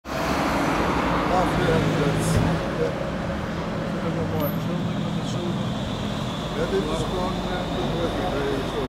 Trolleybussen in Solingen 6.
Hier rijdt trolley 2.0 206 naar het remise terrein waar de open dag werd gehouden ten ere van het 70 jarige bestaan van de trolleybussen in Solingen.